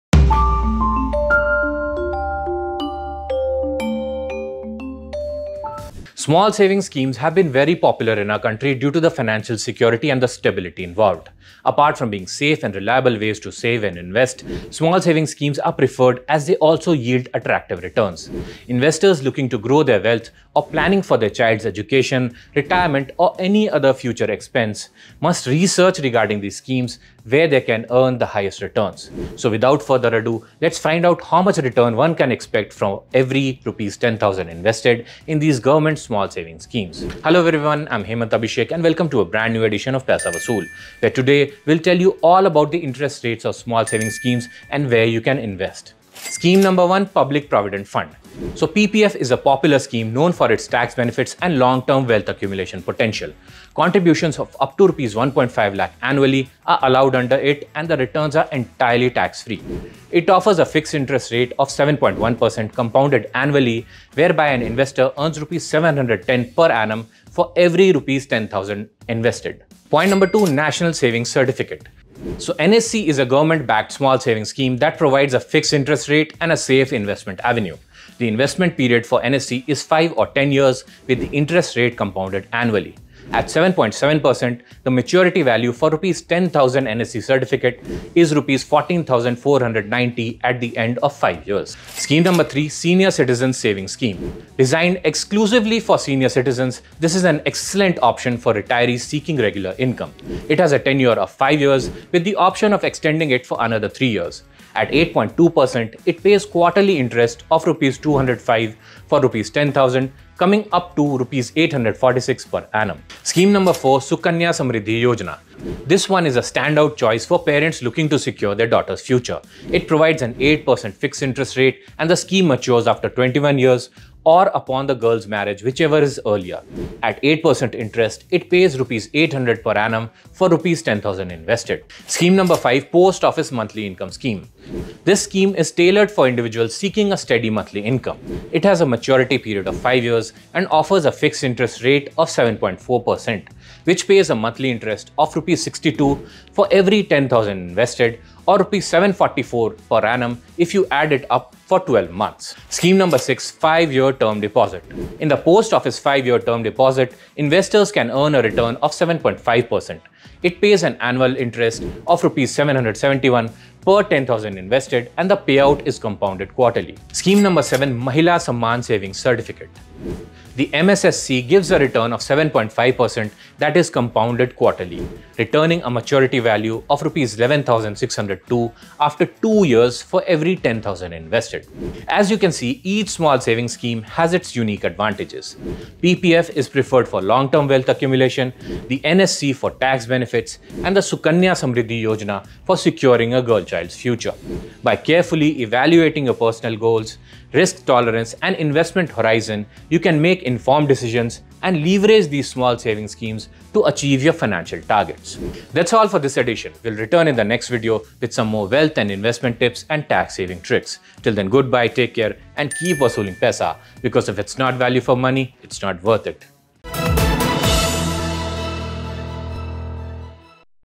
Paisa Wasool 2.0: 7 Best small saving schemes with highest returns
Small savings schemes have been very popular in India due to the financial security and the return they offer. Investors looking to grow their wealth, or planning for their child's education, retirement, or any other future expense can invest in small saving schemes to earn high returns. In this video we discuss the returns that investors can expect for every Rs 10,000 invested in these schemes.